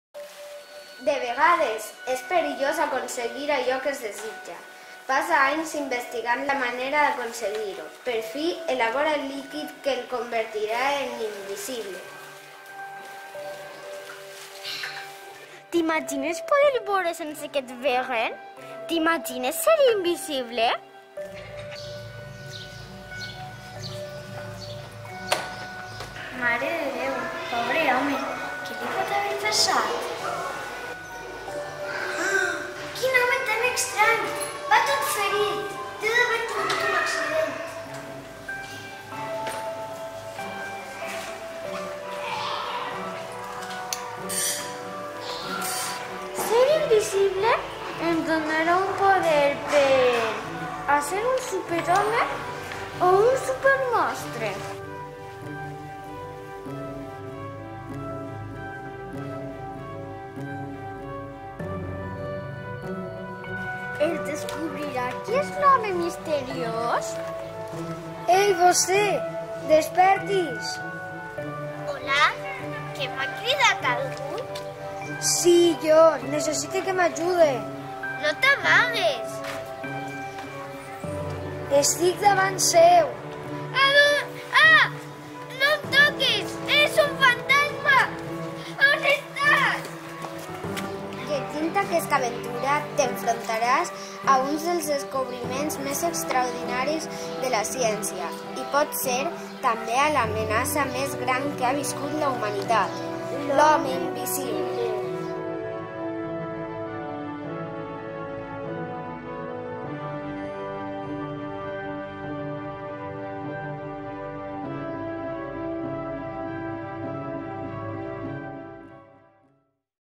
micevalbktpri_11_''L´HOME INVISIBLE''
Alumnat de 3r/3º de primària, grup A del centre CEIP MESTRE RAFAEL NOGUERA de DAIMÚS
De vegades es perillós aconseguir allò que es desitja. Aquesta es l´història de Griffin, un jove científic plé d´ambició i curiositat per elaborar el líquid que e convertirà en invisible. Com serà l´arribada de l´home misteriós a l´escola de Daimús?
A veces se peligroso conseguir aquello que se desea. Esta se l´historia de Griffin, un joven científico lleno de ambición y curiosidad para elaborar el líquido que e convertirá en invisible. Cómo será l´llegada de l´hombre misterioso a la escuela de Daimús?